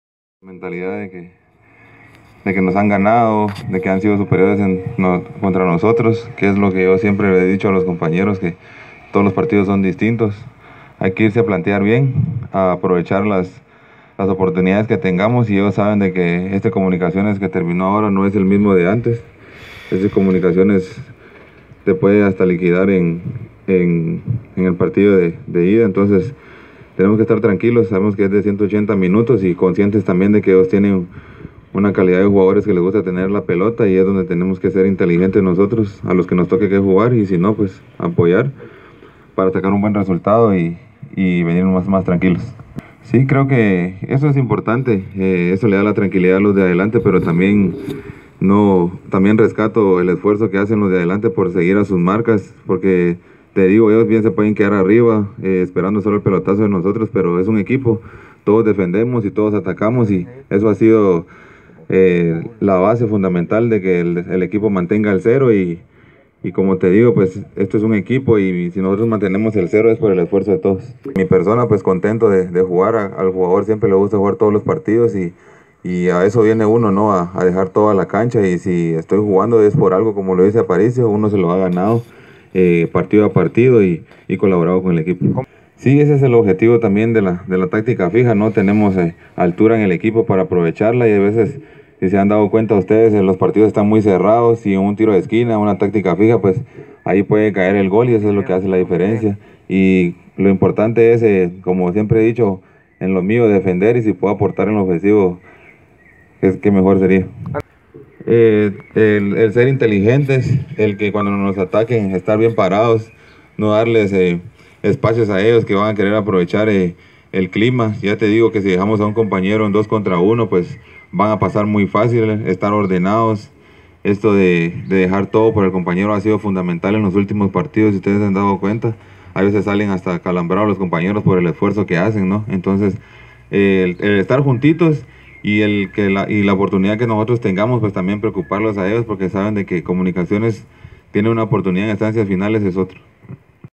Wilson Lalin
El defensor de Comunicaciones habló sobre el encuentro que tendrán el jueves ante los venados.